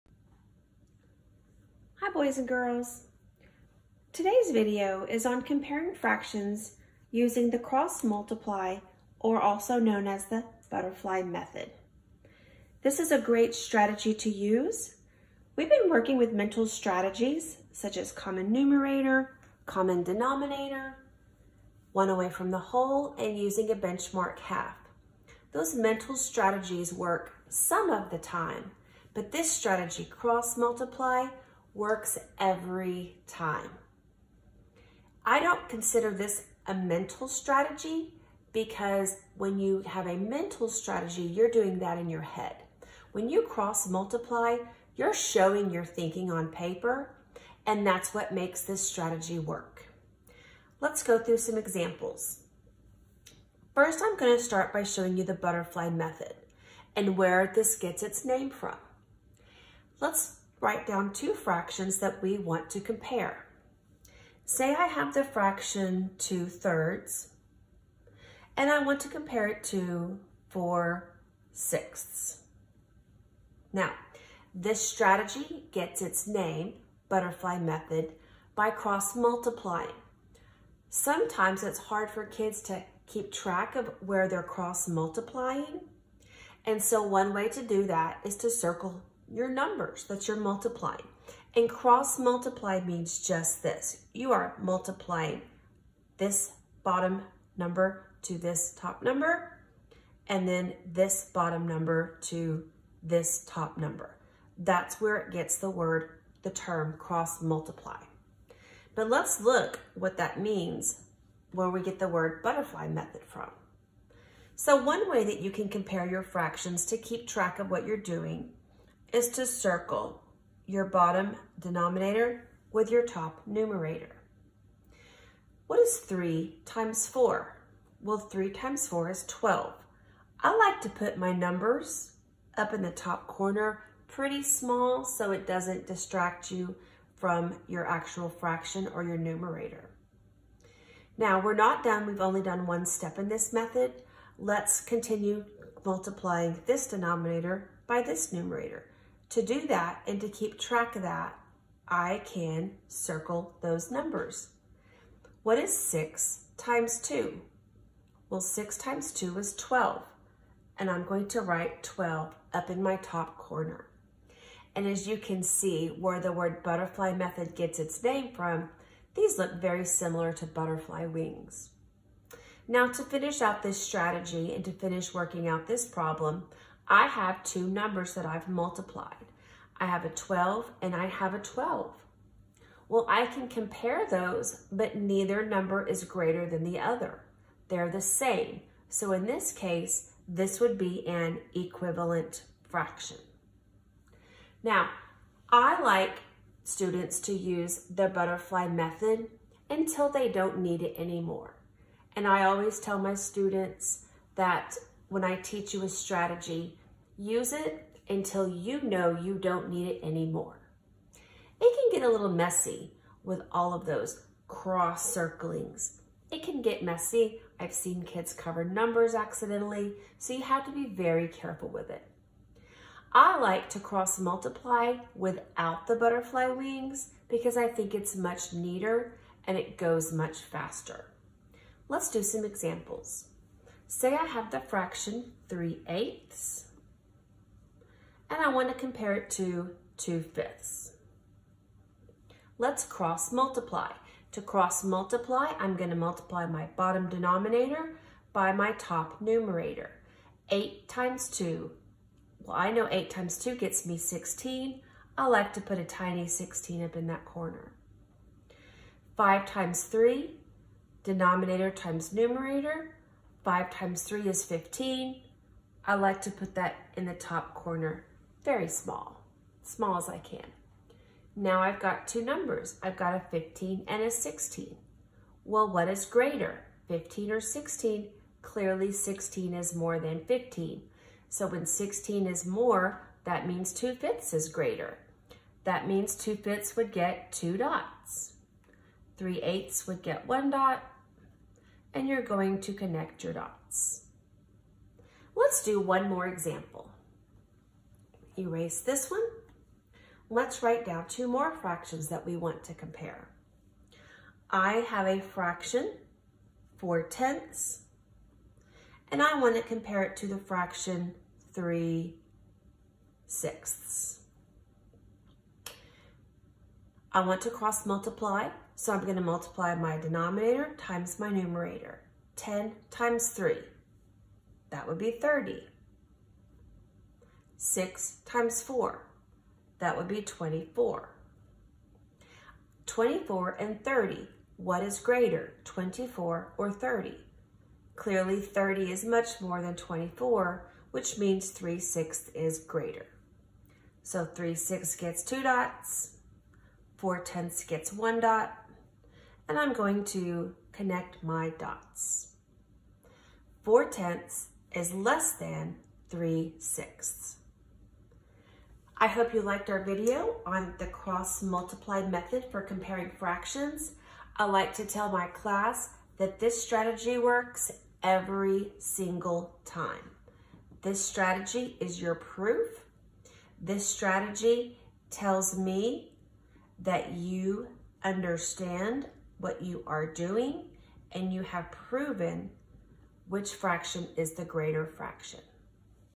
Comparing Fractions with Cross Multiplication
This video shows students how to compare fractions by using the "butterfly method" or Cross multiplication